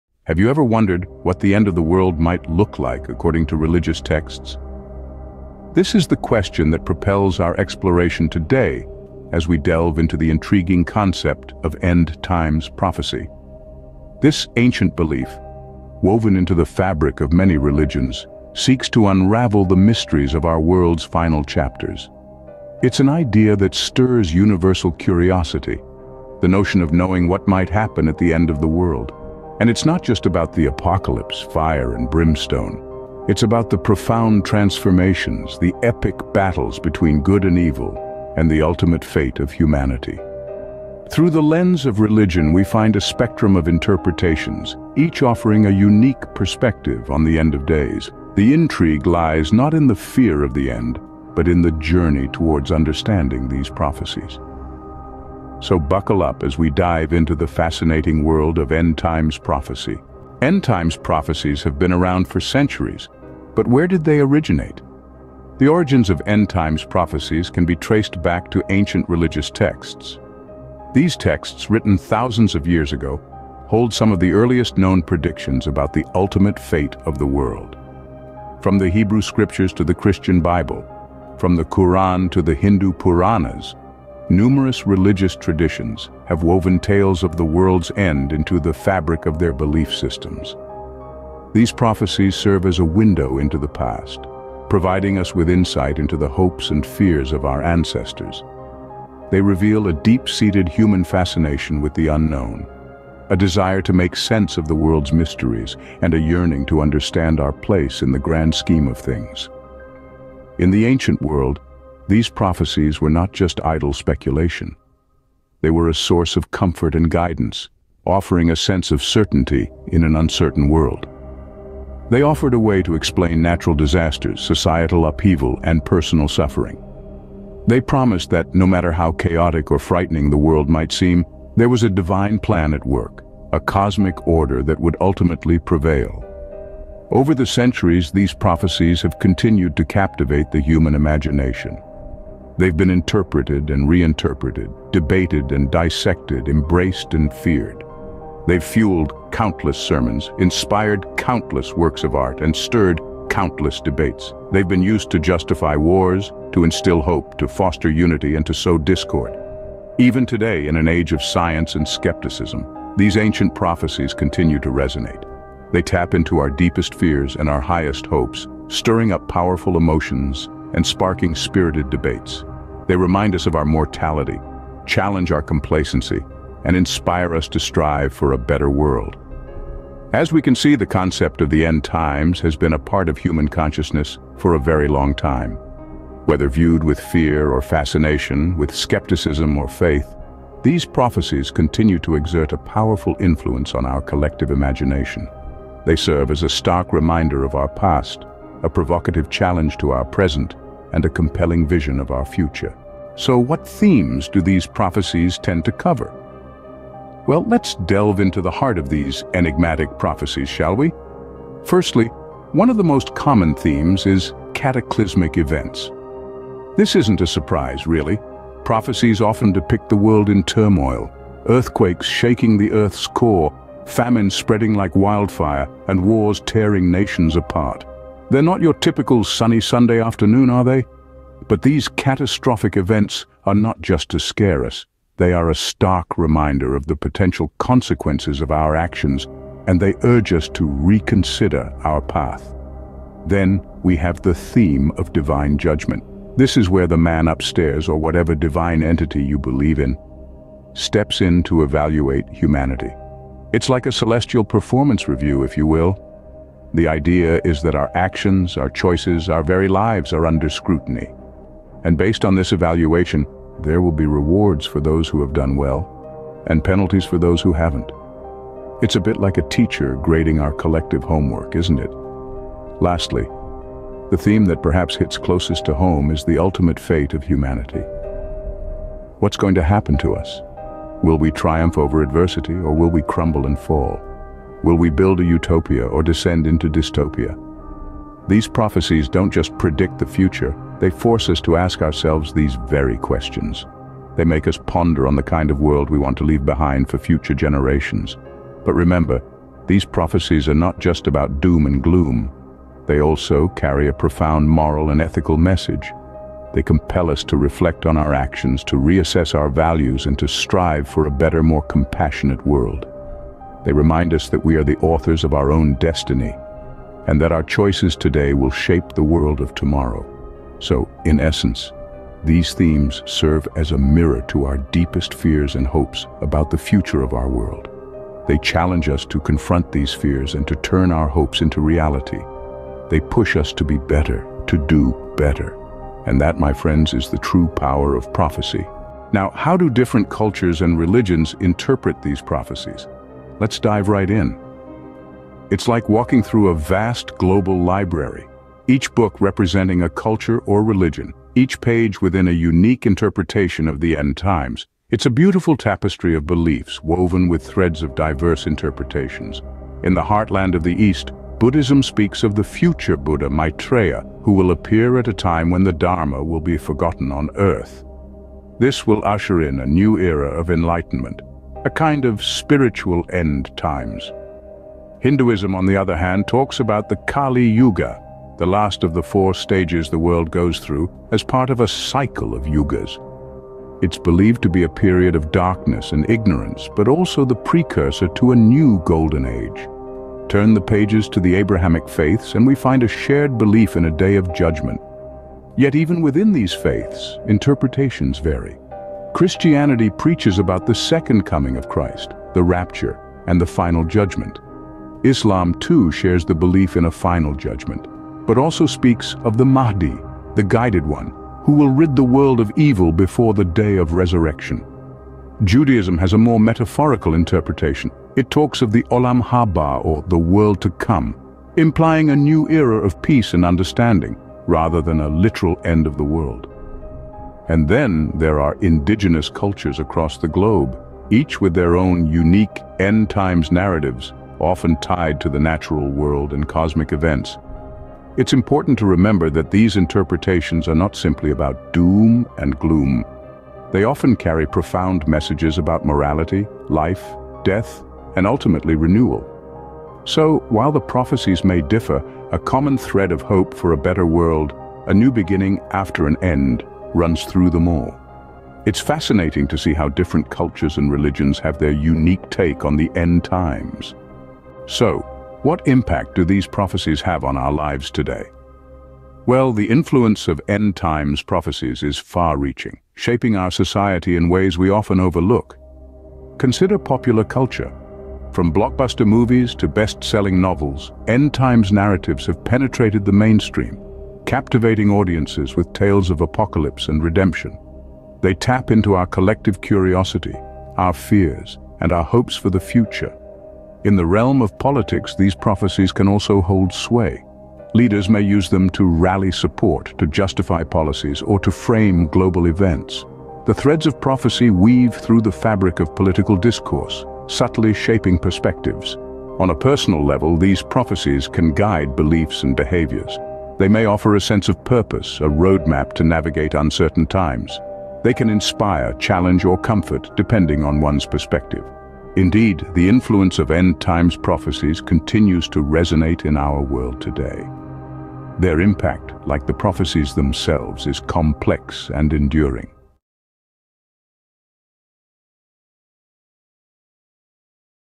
Understanding End Times Prophecy
Embark on a journey of discovery as we demystify the intricate world of end times prophecy in this beginner's guide. Whether you're new to the topic or seeking a refresher, join us as we navigate through the foundational principles, key concepts, and prominent themes found in prophetic scriptures. From the Olivet Discourse to the Book of Revelation, we'll provide an accessible overview of the major prophetic passages and their significance in understanding the end times. Whether you're a skeptic or a seeker, this video offers a welcoming introduction to the fascinating realm of biblical prophecy. Tune in to gain a clearer understanding of end times prophecy and its relevance for today's world.